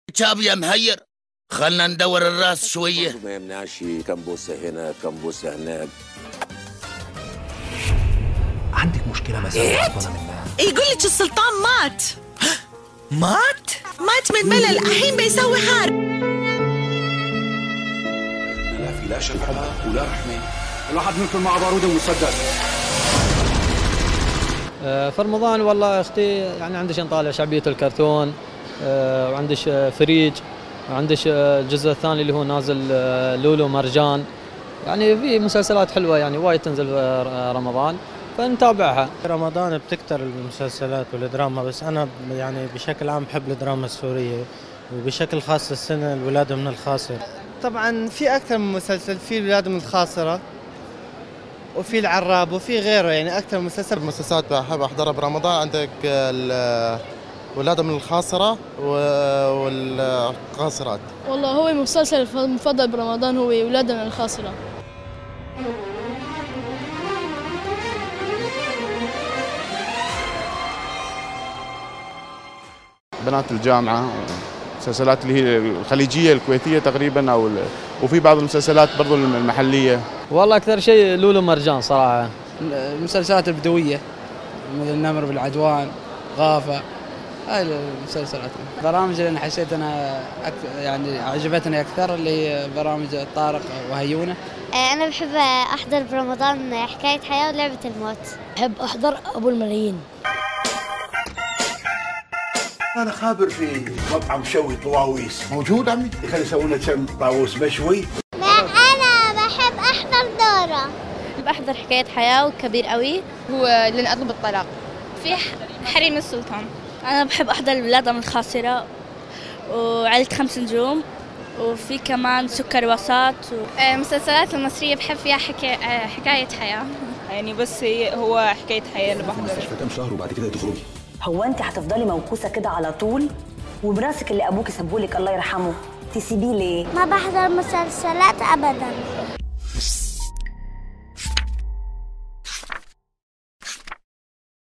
ماذا يتابع المشاهدون في رمضان؟
ما بين مسلسلات مصرية وأخرى خليجية وثالثة سورية، يجلس المشاهدون في رمضان لمتابعة الأعمال الدرامية التي تتسابق على الاستحواذ على وقت المشاهد في رمضان، هنا استطلاع عن أهم الأعمال الدرامية في رمضان.